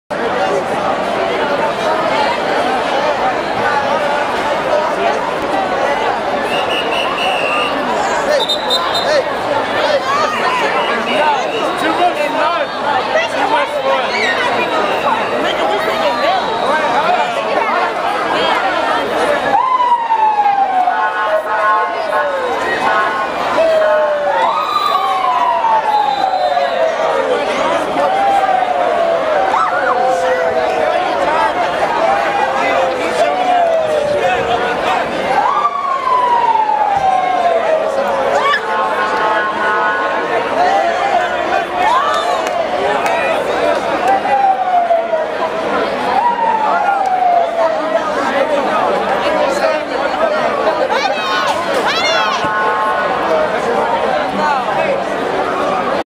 Austin Police Riding Horses Through the Crowd, 6th Street, 12/15/12
Videography  by Lynn, of the Peaceful Streets Project.